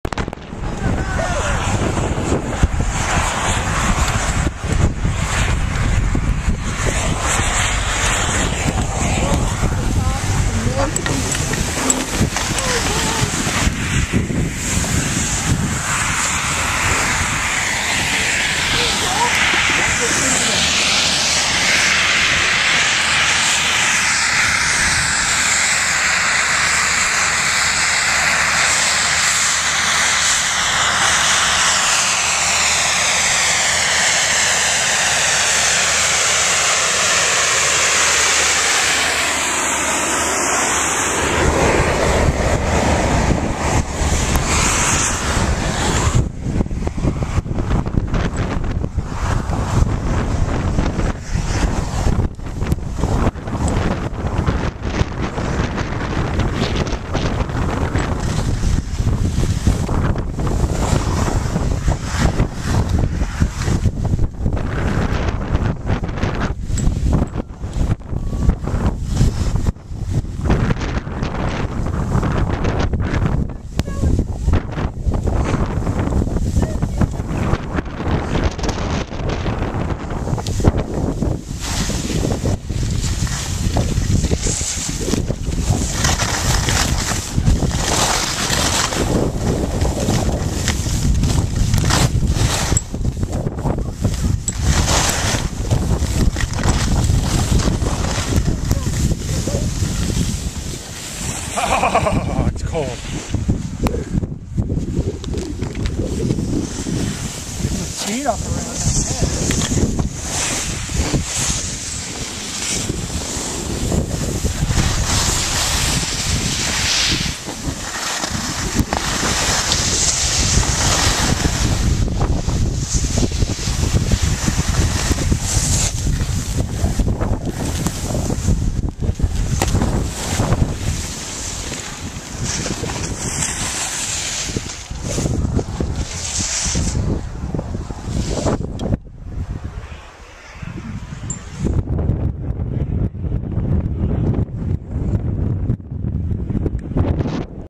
Whiteface Mountain and New York State Skiing
Late December 2012.  Absolute whiteout on 4,386 foot summit of New York State's Whiteface Mountain.
 This is why they call it Whiteface.